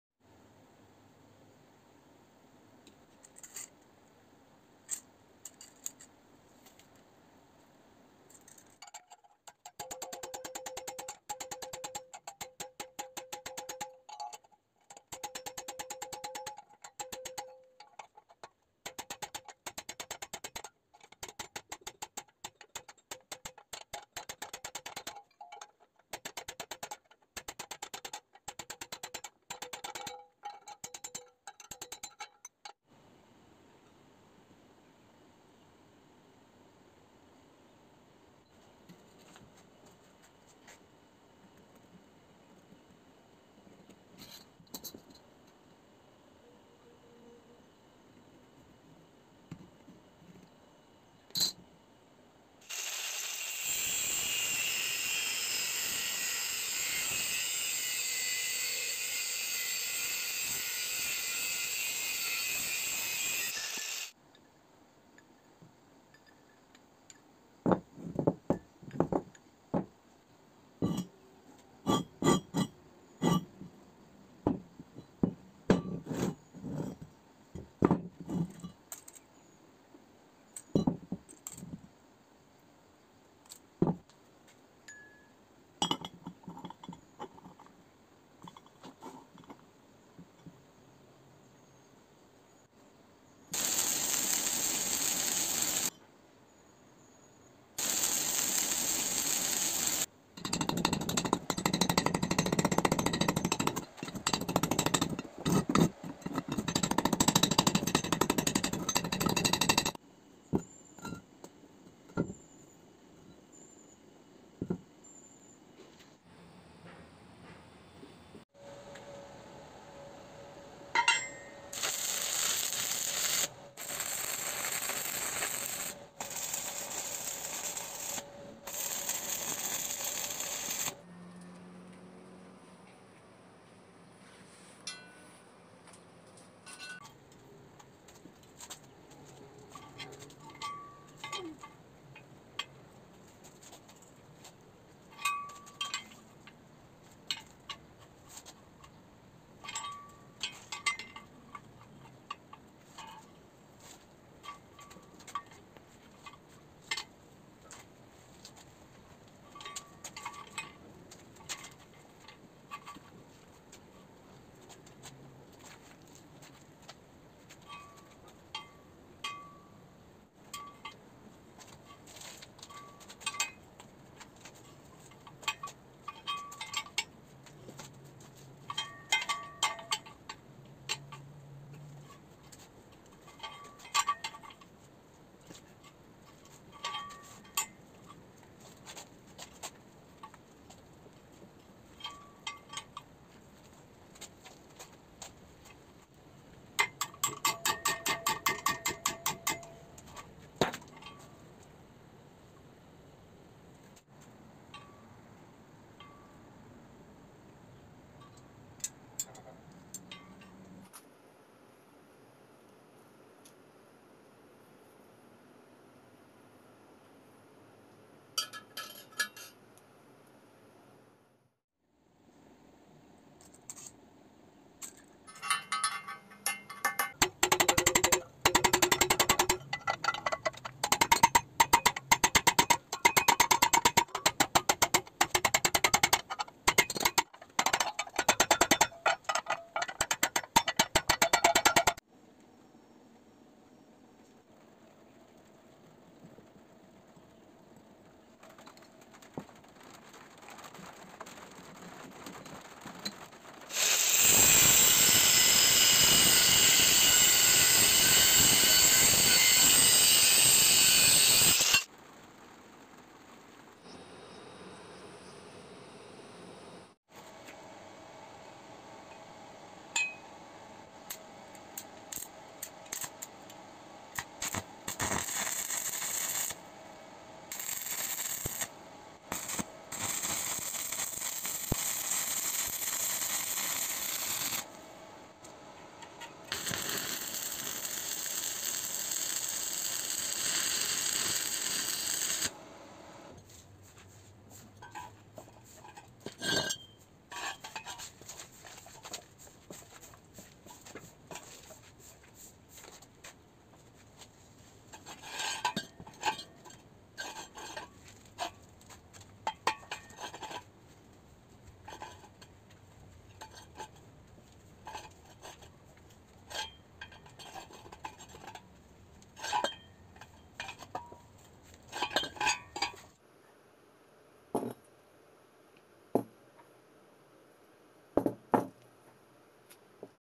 Discovery of an iron bending tool rarely known by welders
Useful ideas for tricks for bending metal bars for flat bar

Boy let's make a metal bender

Another useful tool

Hello friends, welcome to the mastermind family